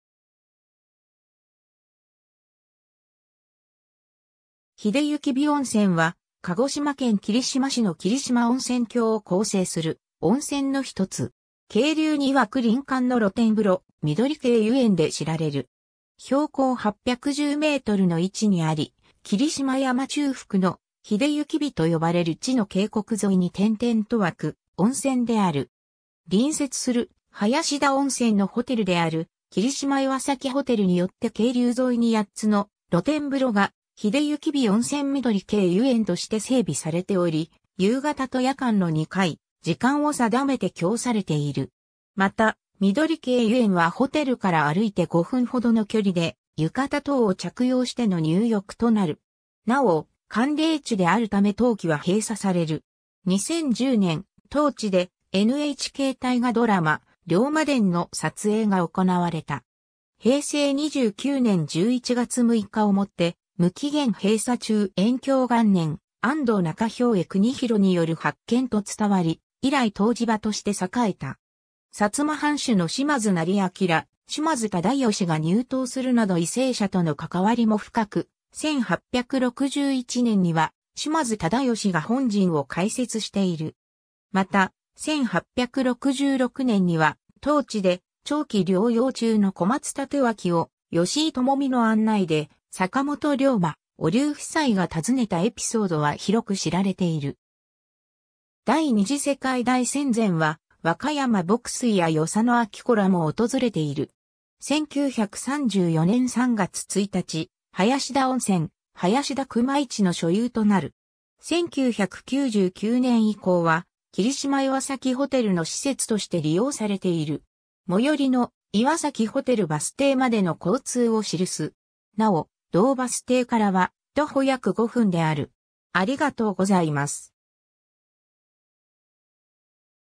栄之尾温泉
栄之尾温泉（えいのおおんせん、えのおおんせん）は、鹿児島県霧島市（旧国大隅国）の霧島温泉郷を構成する温泉の一つ。
渓流に湧く林間の露天風呂、緑渓湯苑で知られる。
 標高810メートルの位置にあり、霧島山中腹の「栄之尾」と呼ばれる地の渓谷沿いに点々と湧く温泉である。
隣接する林田温泉のホテルである「霧島いわさきホテル」によって渓流沿いに8つの露天風呂が「栄之尾温泉 緑渓湯苑」として整備されており、夕方と夜間の2回、時間を定めて供されている。
また、緑渓湯苑はホテルから歩いて5分ほどの距離で、浴衣等を着用しての入浴となる。
なお、寒冷地であるため冬季は閉鎖される。
 2010年（平成22年）、当地でNHK大河ドラマ「龍馬伝」の撮影が行われた。
 平成２９年１１月６日をもって無期限閉鎖中 延享元年（1744年）、安藤仲兵衛国広による発見と伝わり、以来湯治場として栄えた。
 薩摩藩主の島津斉彬、島津忠義が入湯するなど為政者とのかかわりも深く、1861年（文久元年）には島津忠義が本陣を開設している。
また、1866年（慶応2年）には当地で長期療養中の小松帯刀を吉井友実の案内で坂本龍馬・お龍夫妻が訪ねたエピソードは広く知られている。
 第二次世界大戦前は、若山牧水や与謝野晶子らも訪れている。
 1934年（昭和9年）3月1日、林田温泉・林田熊一の所有となる。
 1999年（平成11年）以降は、霧島いわさきホテルの施設として利用されている。
 最寄りの「いわさきホテル」バス停までの交通を記す。
なお、同バス停からは徒歩約5分である。